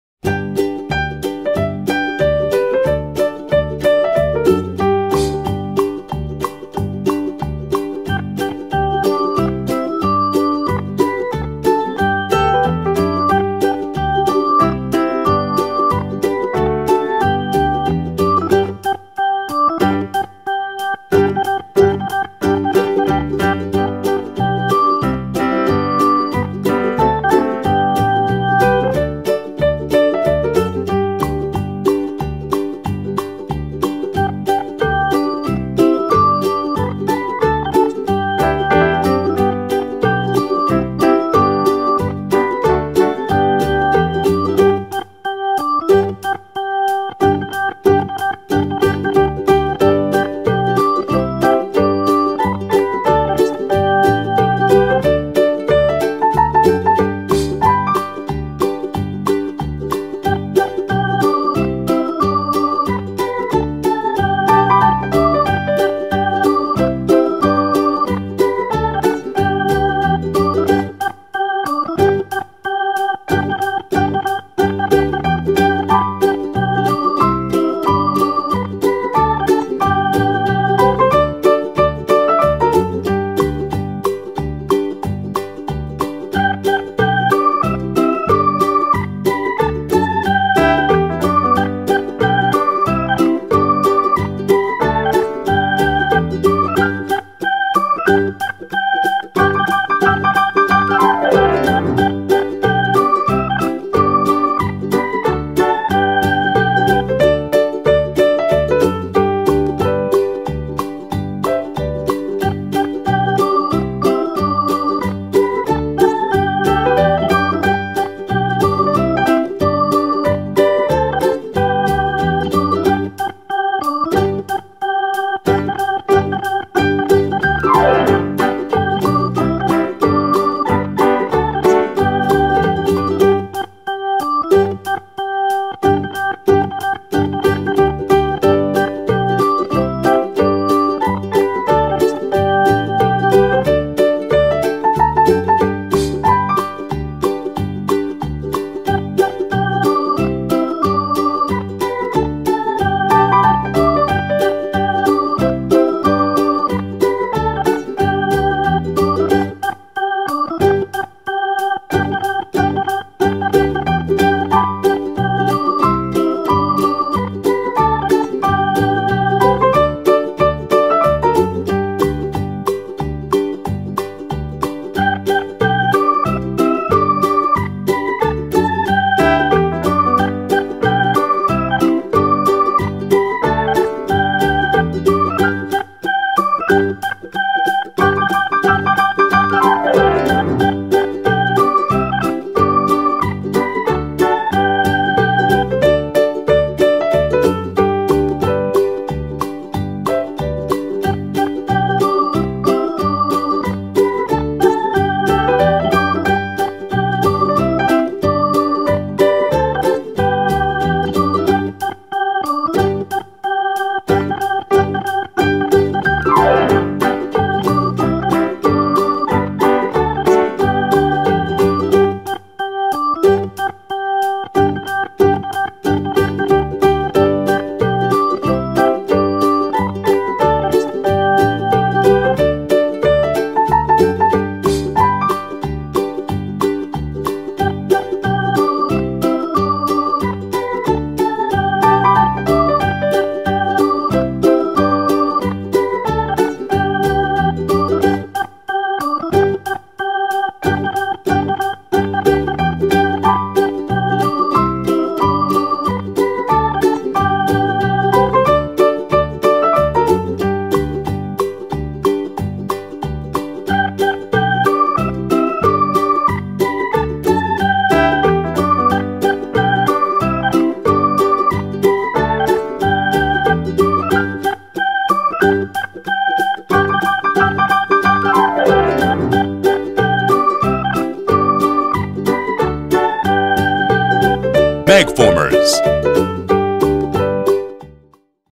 2016 NEW_MAGFORMERS SPACE EPISODE SET
Magformers Space Episode Set comes with astronaut and alien figures together with carious accessories related to space. What kind of models do earthing and aliens make with the space theme? Try to make detailed space models with antenna, wing cover and dome accessories, children can experience a journey into mysterious space.